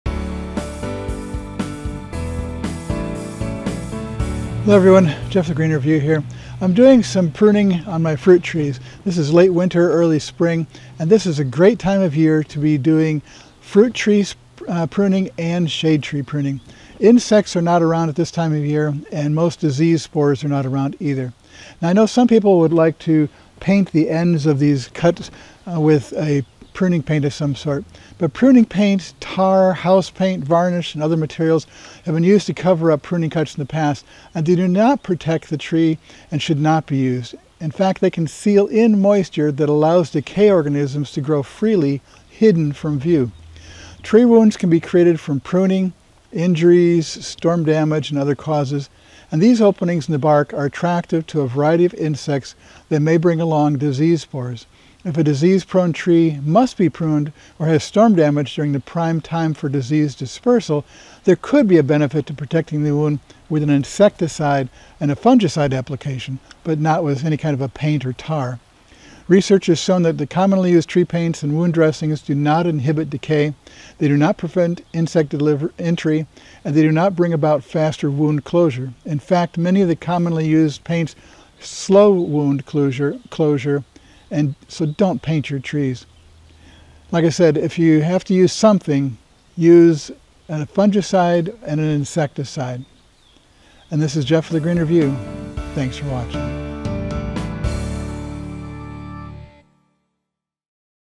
10 24 Pruning Paint?
Pruning paint, tar, house paint, varnish, and other materials have been used to cover pruning cuts. They do not protect the tree and should not be used. In fact, they can seal in moisture that allows decay organisms to grow freely, hidden from view.